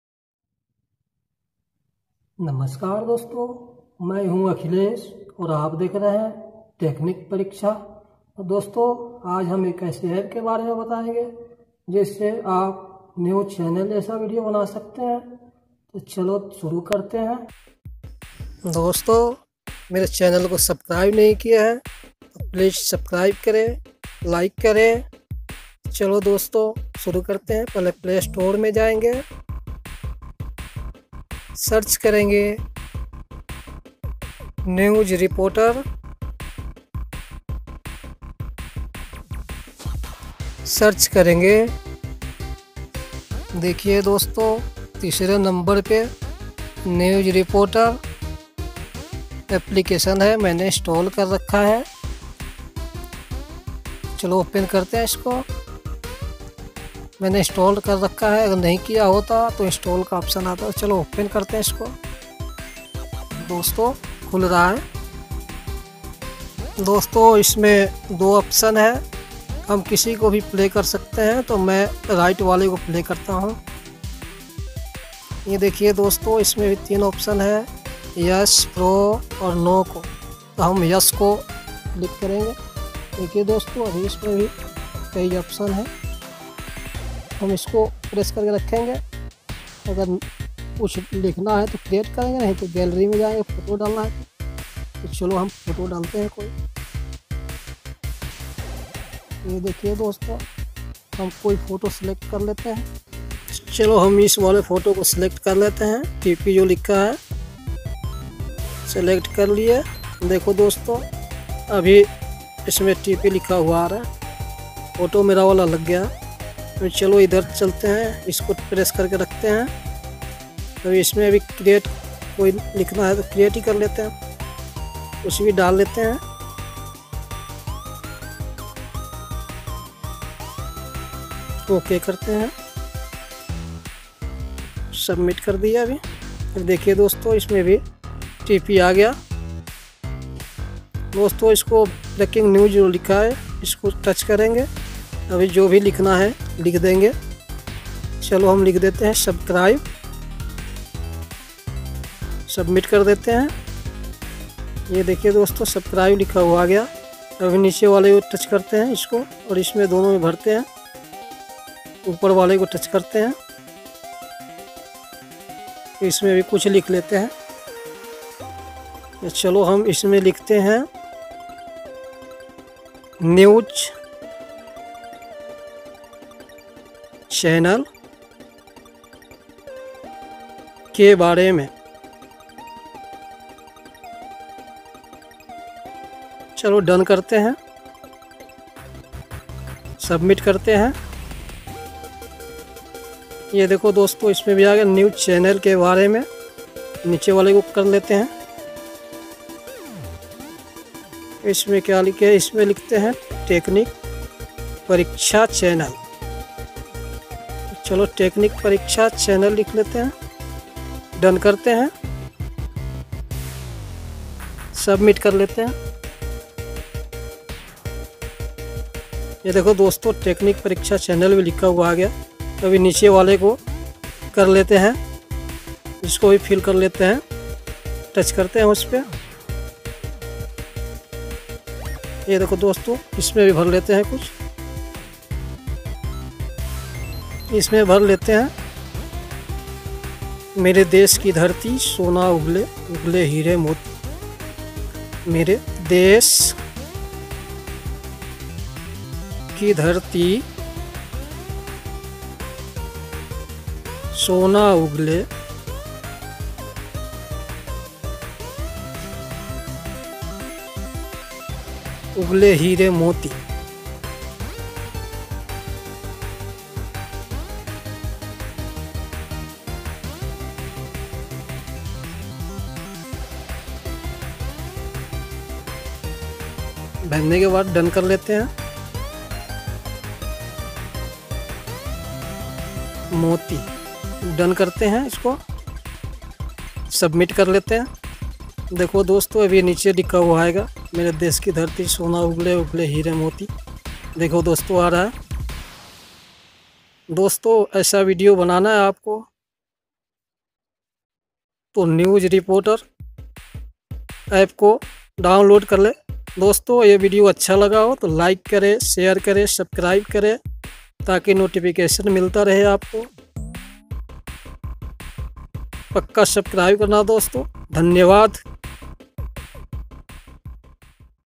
News broadcast video editer/Recommended for you
Hello doston aaplog kaise hain aap log ko Technic Pariksha me swagat hai. Main Akhilesh kumar aaplogon ka swagat karta hoon. Doston aaj mai bataunga news channel jaisa video recording karna to doston playstore मे jayen aur download karlen news reporter applications ko  aur open karke banaye news channel jaisa video doston agar Mere channel abhi tak subscribe nahi kiya hai to subcribe kare like kare aur share kare.